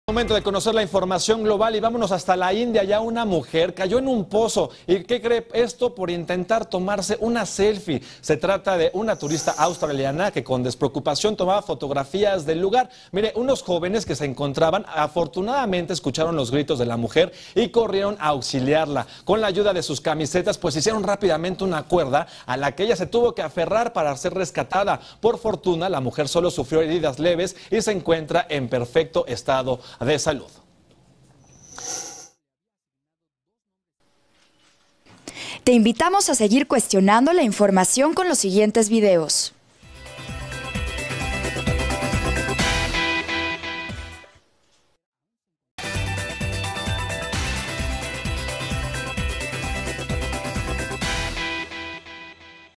Mujer cae a pozo en la India al tratar de tomarse una selfie/ Hiram Hurtado
Una turista australiana cayó a un pozo en la India mientras se hacía un selfie. 28 enero de 2016

Conductor: Hiram Hurtado.
Horario: Lunes a Viernes @10:30-11:00.